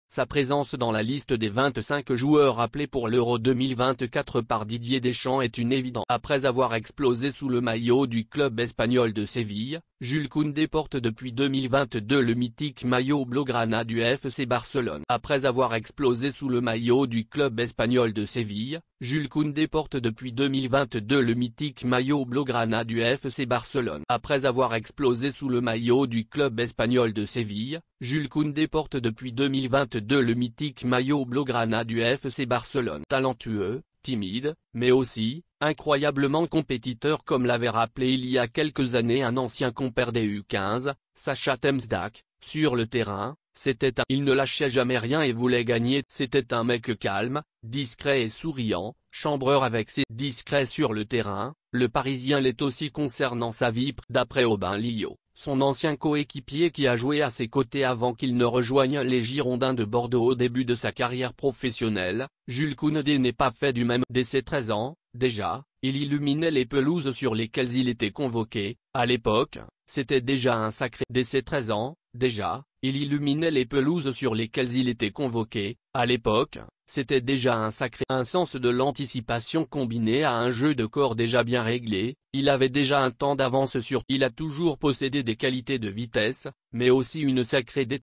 Jules Koundé est-il en couple ?
,jules kounde red card france vs bosnia,koundé skills,jules kounde red card,jules koundé barça,why man utd should sign jules kounde

Jules Koundé est-il en couple ? Sa présence dans la liste des 25 joueurs appelés pour l'Euro 2024 par Didier Deschamps est une évidence... Après avoir explosé sous le maillot du club espagnol de Séville, Jules Koundé porte depuis 2022 le mythique maillot blaugrana du FC Barcelone. Un club prestigieux au sein duquel il a réussi à se faire une place de choix. Talentueux, timide, mais aussi, incroyablement compétiteur comme l'avait rappelé il y a quelques années un ancien compère des U15, Sacha Temsdak : Sur le terrain, c'était un soldat. Il ne lâchait jamais rien et voulait gagner tous les combats. C'était un mec calme, discret et souriant, chambreur avec ses intimes. Discret sur le terrain, le parisien l'est aussi concernant sa vie privée. D'ailleurs, est-il un cœur à prendre ? Pour le moment, aucune preuve ne laisse penser que Jules Koundé est en couple, que ce soit sur son compte Instagram où dans les gradins : pas de wag dans les parages. D'après Aubin Liot, son ancien coéquipier